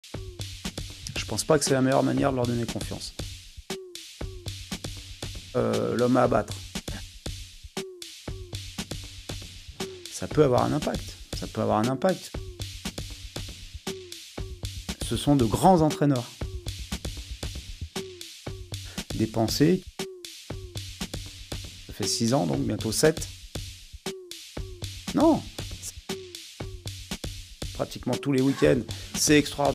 Interview courte avec Bruno Génésio
Voici enfin l'interview courte de Bruno Génésio enregistrée pour PST Sport en mai dernier!